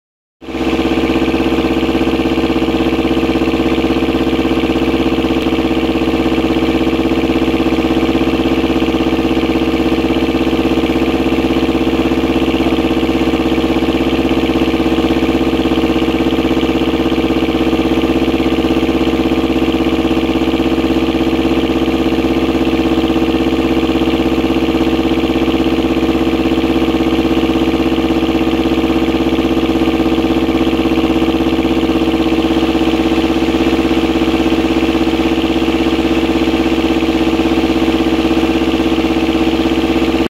SPAL fan relay kit 1
SPAL fan relay kit used to cool Yanmar 2TNV70.  ON at 185F (+85C), OFF at 165F (+74C)